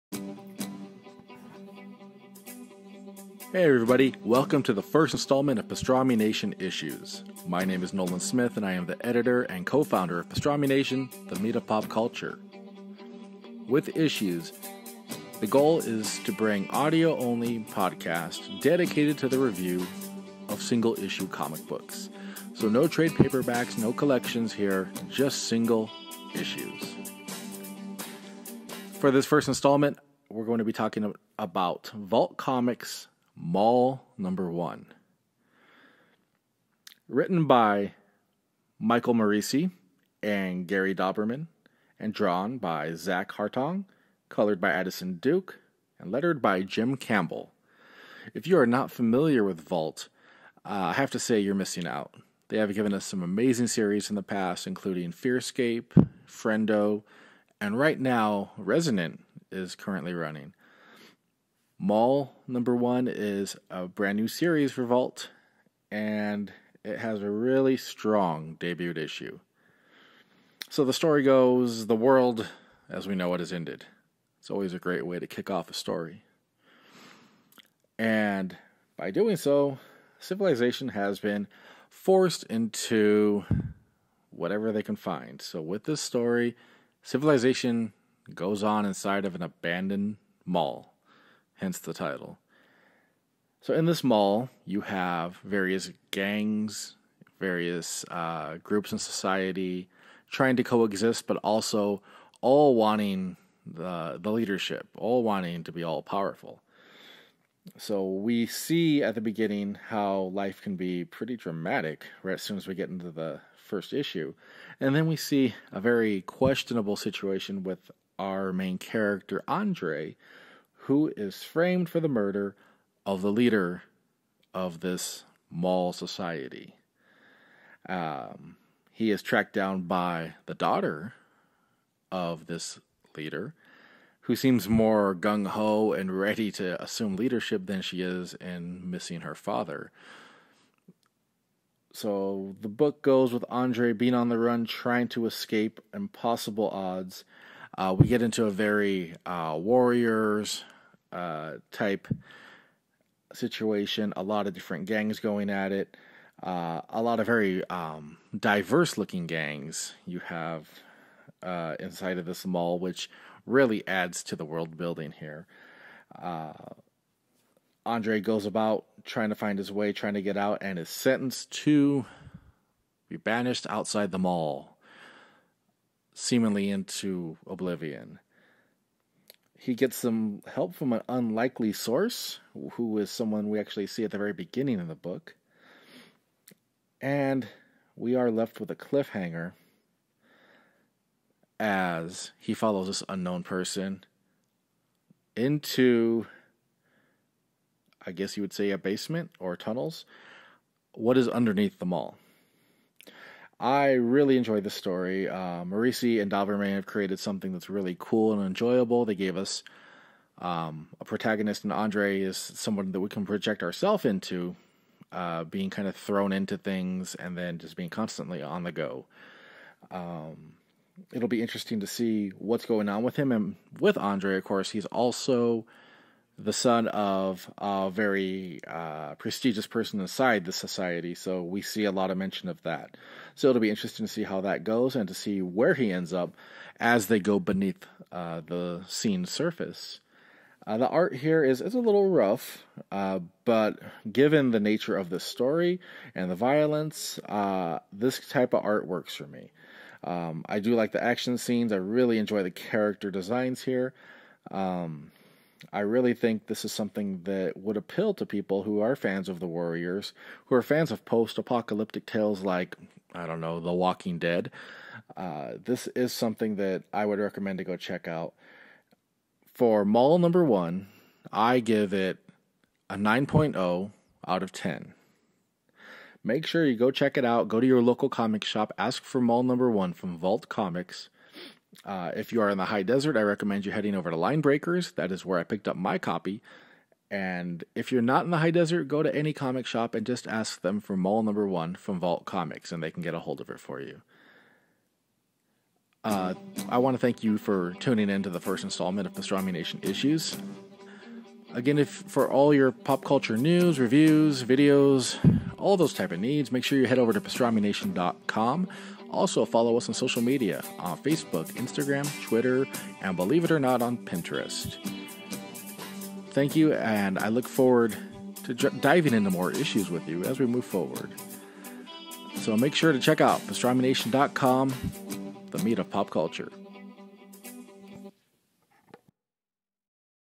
Pastrami Nation Issues Mall #1
Welcome to the first installment of Pastrami Nation Issues! Our audio only podcast dedicated to the review of single issue comic books! For the very first installment, Pastrami Nation Editor and Co-Founder Nolan P. Smith review Vault Comics Mall #1!   Make sure to visit PastramiNation.com for news, reviews, podcasts, videos, giveaways and more!

Music